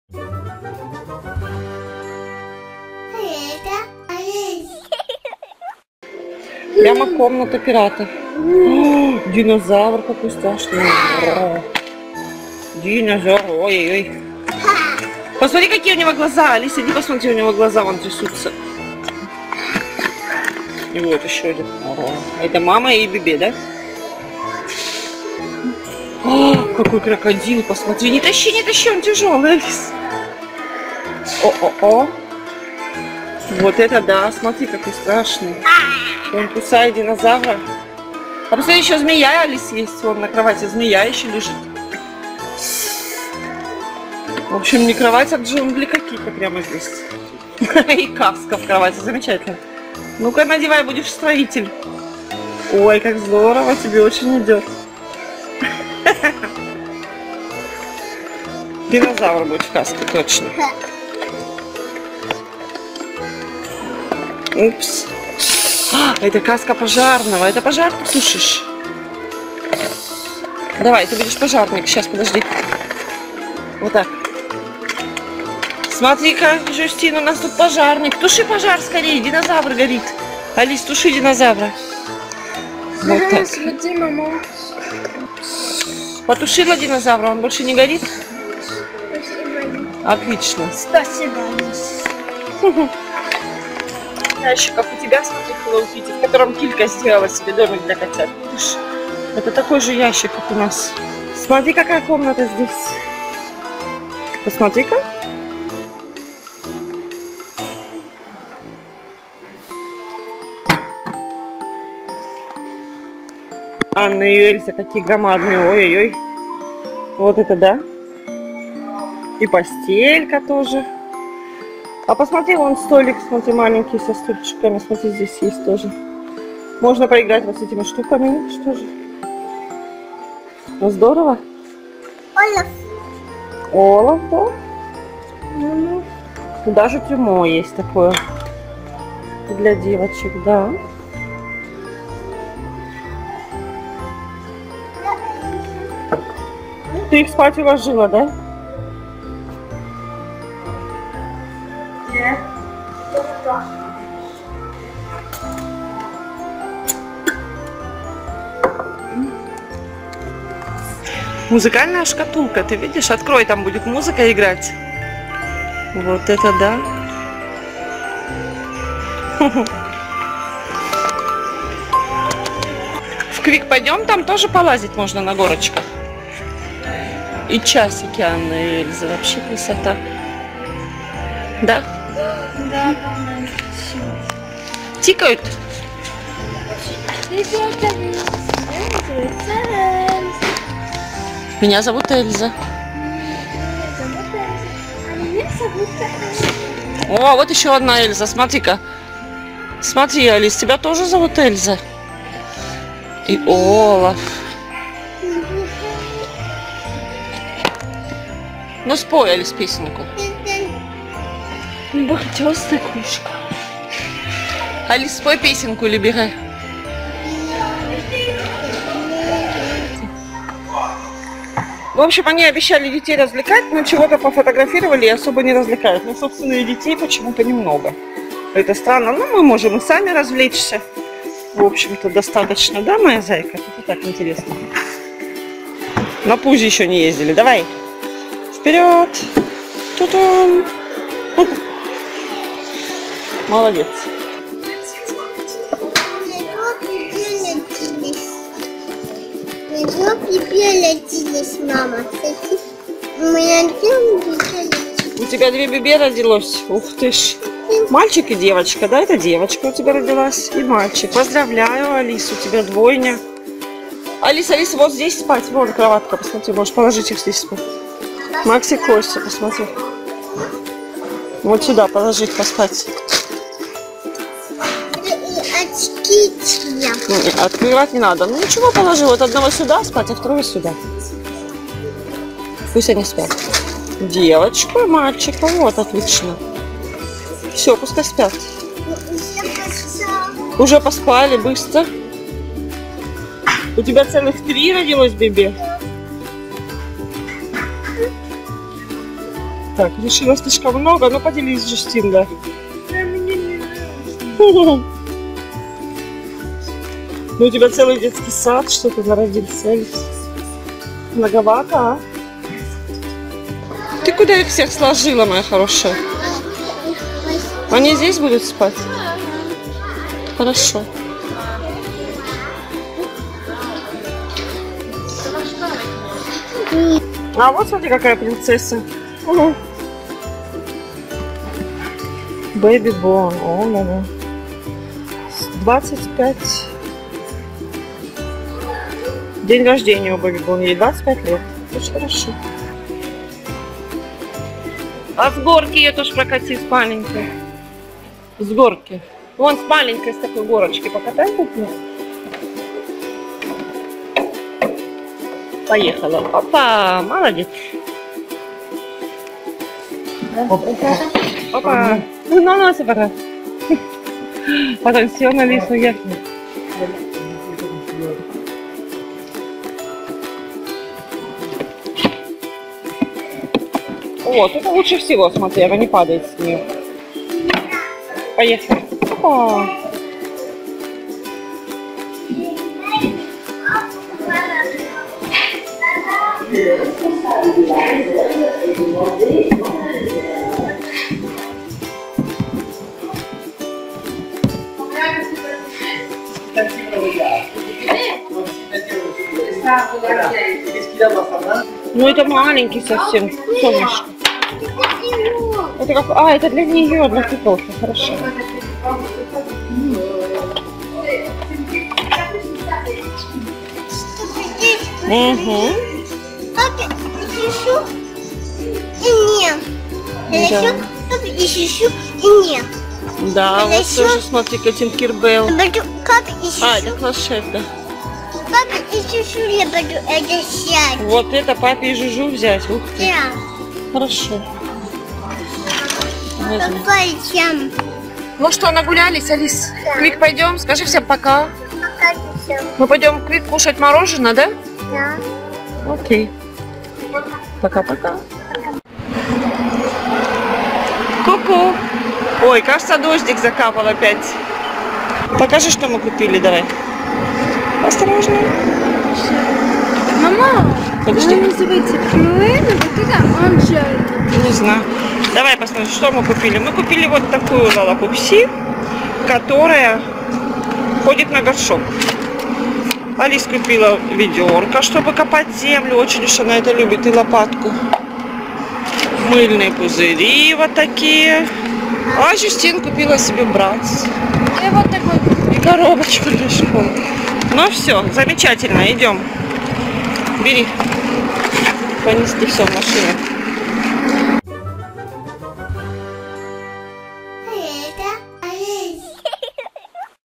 🐻 Магазин игрушек//Детская комната Принцессы//Играем с куклой /2-часть
Алис играет с куклой, смотрим детские комнаты - Принцессы для девочек и комнату для мальчиков с динозавром:)
Алис принимает участие в мероприятии магазина игрушек "Dreamland" - День на свежем воздухе. По случаю плохой погоды магазин игрушек устроил детскую площадку не на улице, а прямо внутри магазина:) Играем с игрушками, катаемся с горки, на качелях, катаем кукол Беби Борн на качелях, горках и велосипедах:)